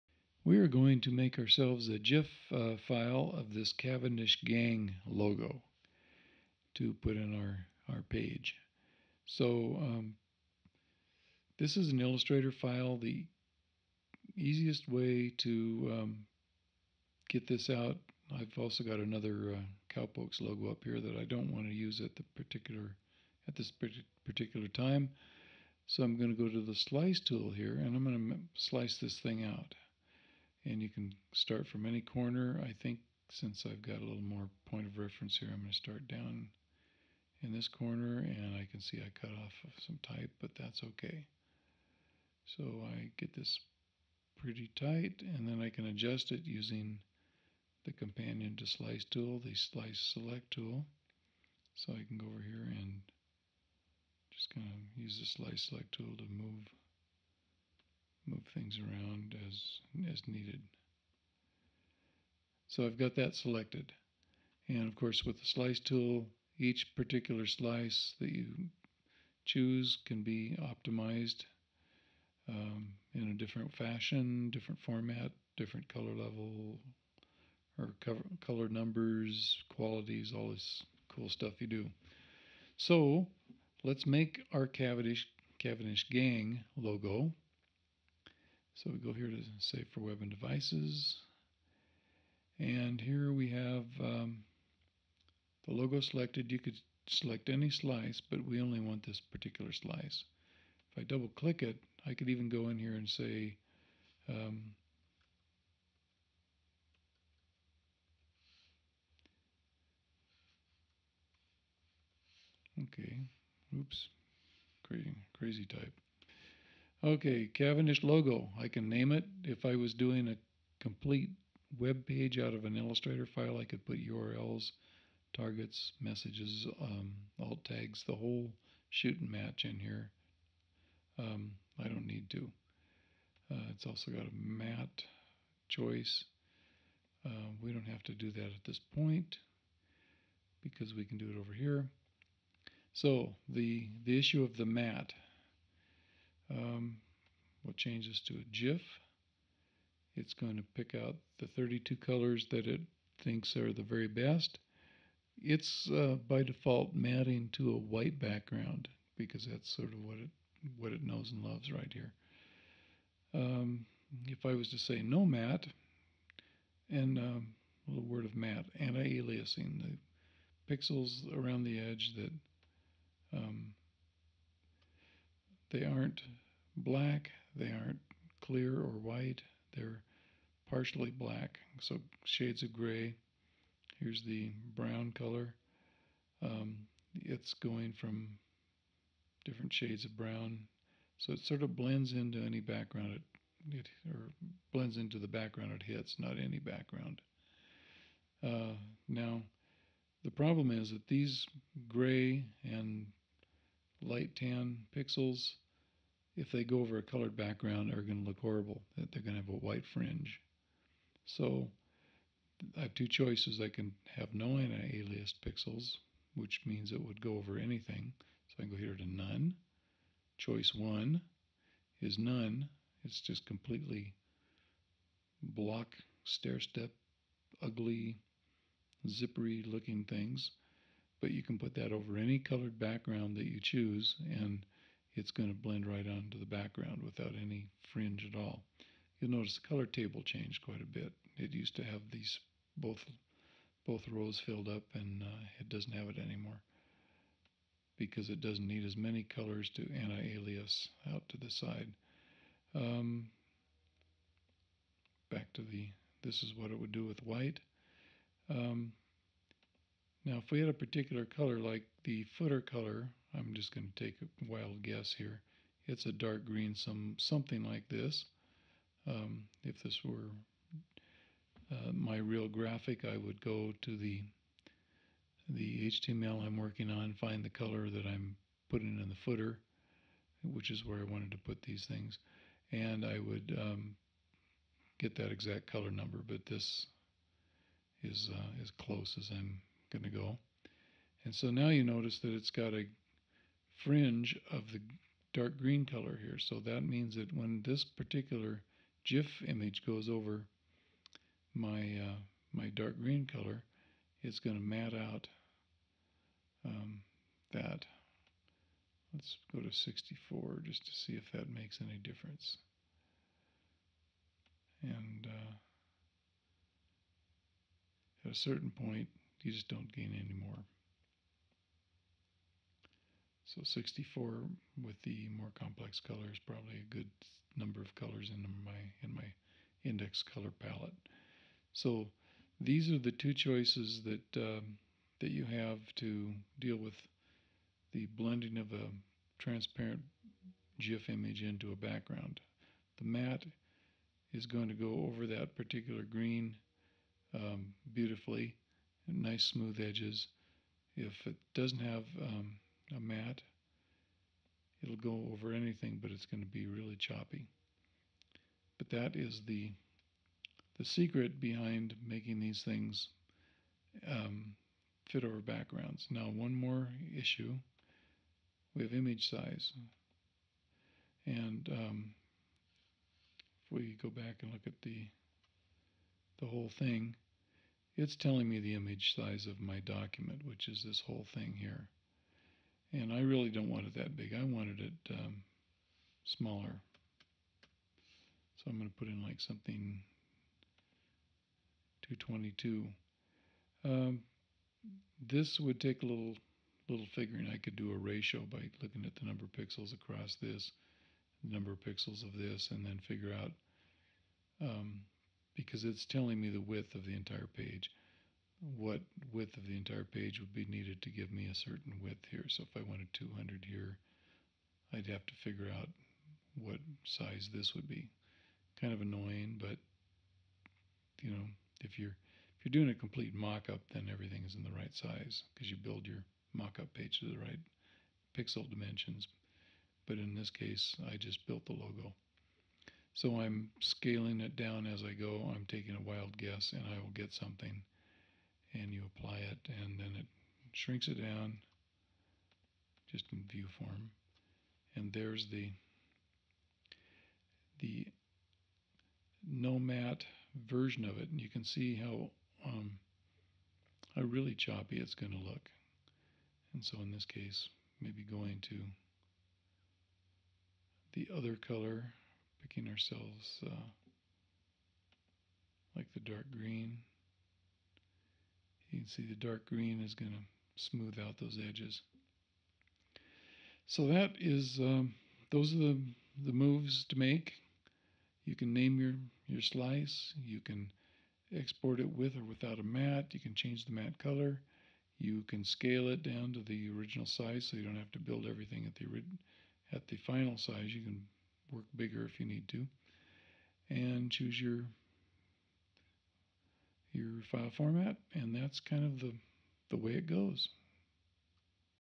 Matte edges on GIF web graphics
Here's a look at the matte controls in Save for Web and Devices. A little slicing technique, too. A look at what a matte does and how to choose the right color for your final use.